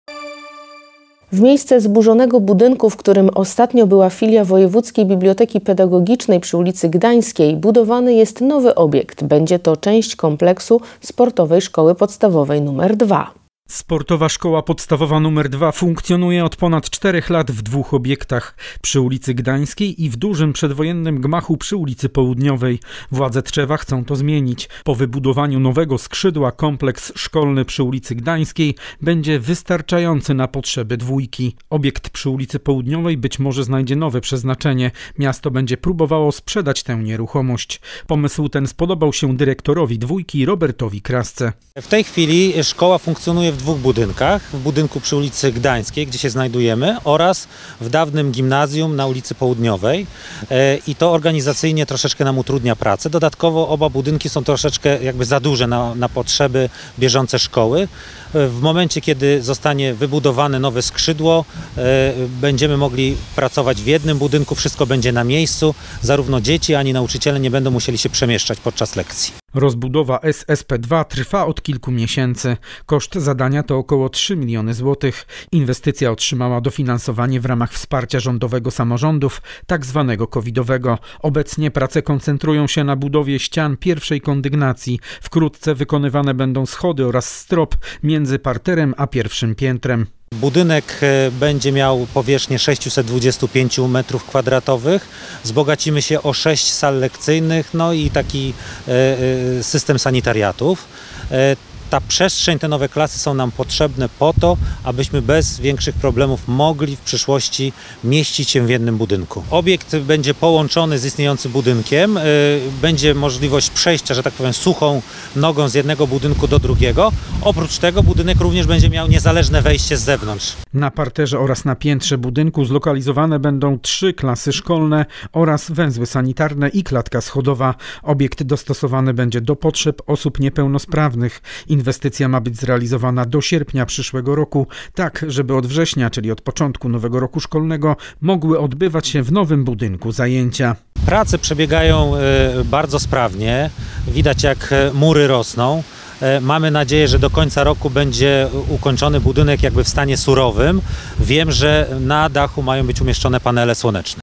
Budują skrzydło dwójki - Tv Tetka Tczew HD
W miejsce zburzonego budynku, w którym ostatnio była filia Wojewódzkiej Biblioteki Pedagogicznej przy ulicy Gdańskiej budowany jest nowy obiekt. Będzie to część kompleksu Sportowej Szkoły Podstawowej numer 2.